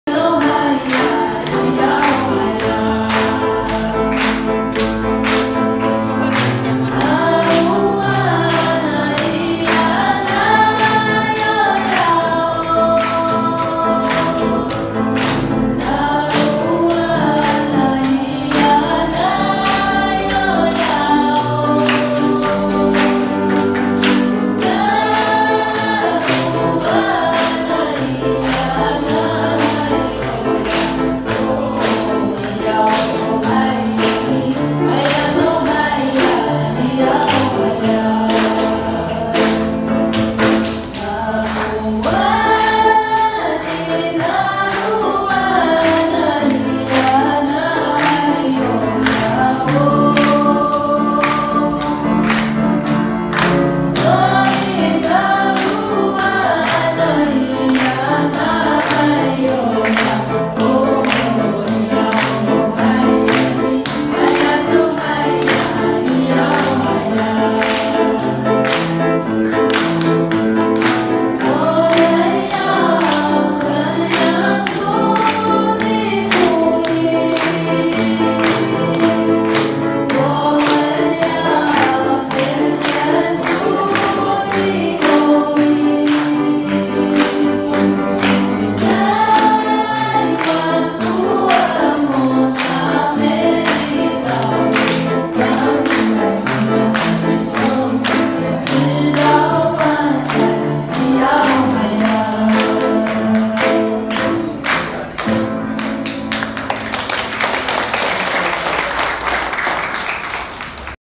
高士教會讚美
車城教會第二任牧師楊昆霖牧師授職禮拜